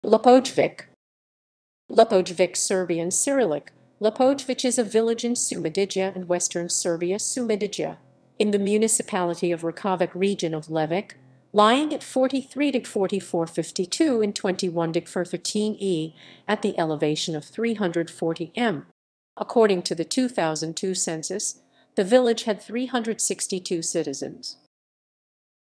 Lepojević
Lepojević (Serbian Cyrillic: Лепојевић) is a village in Šumadija and Western Serbia (Šumadija), in the municipality of Rekovac (Region of Levač), lying at 43°44′52″N 21°04′13″E, at the elevation of 340 m. According to the 2002 census, the village had 362 citizens.